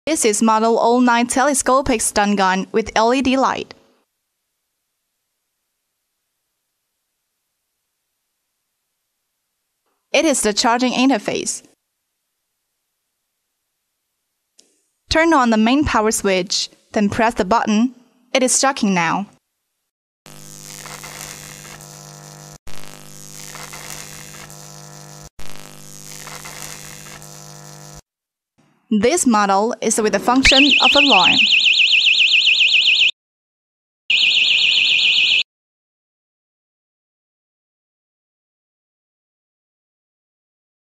TW-09 Heavy Duty Expandable Stun Baton with High-decibel Electronic Alarm
TW-09 stun gun really packs a punch! Just test firing this unit into the air is often enough to stop an attacker. As the bright electric current pulsates between the test prongs and creats an intimidating electrical sound, an attacker with any sense at all will be stopped in his tracks If the sight and sound doesn't stop him, a jolt from this stun gun certainly will bring an attacker down! When an attacker is touched by the stun gun it will disrupt the message the brain sends to the voluntary muscles. Simply touching an attacker will deliver a high voltage shock causing loss of balance and muscle control, confusion, and disorientation bringing him to his knees and making him incapable of further aggressive activity. Due to restrictions in some countries and regions, the customer is responsible for proper use, storge and knowledge of local laws and regulations regarding ownership and usage of this product.